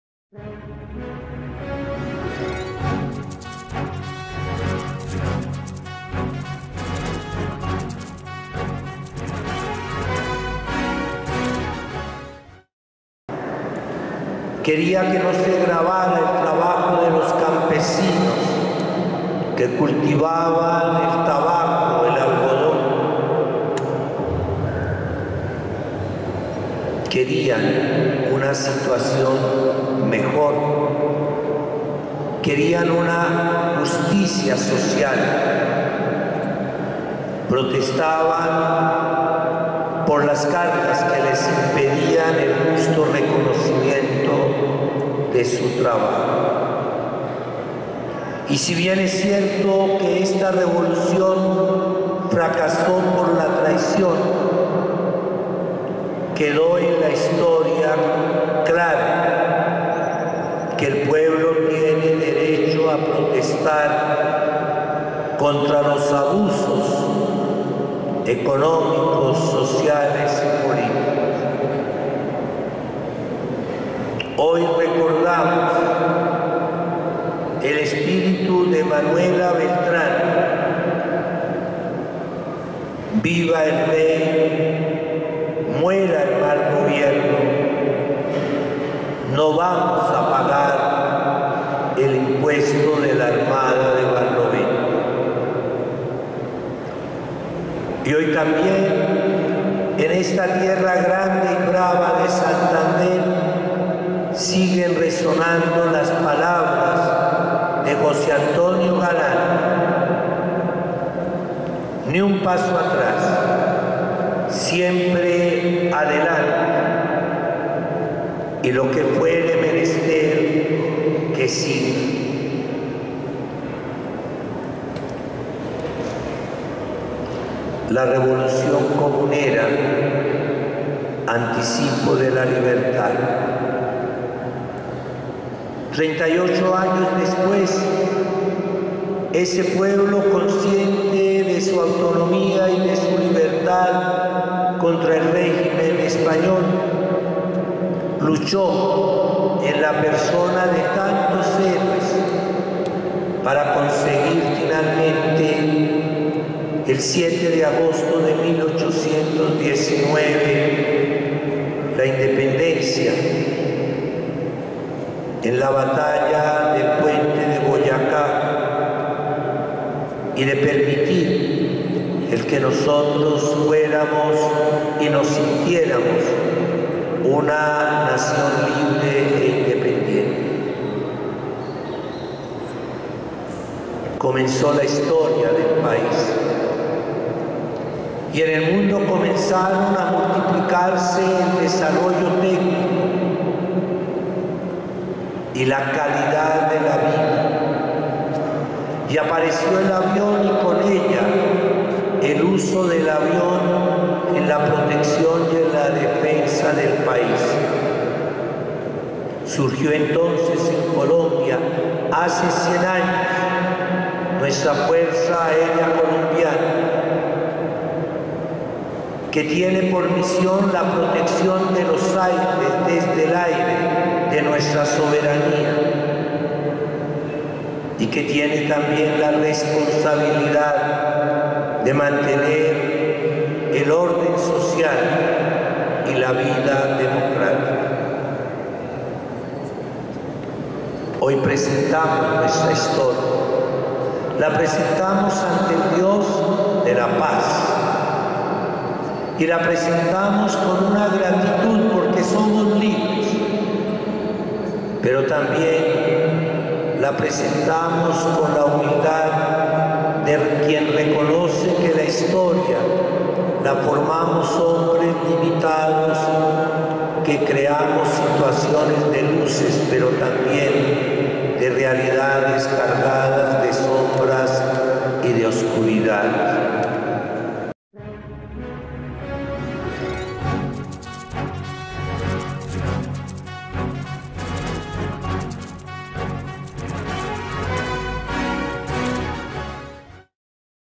Celebracion 238 años de insurrección comunera en el Socorro homilia
El Municipio del Socorro conmemoró 238 años de la Revolución Comunera.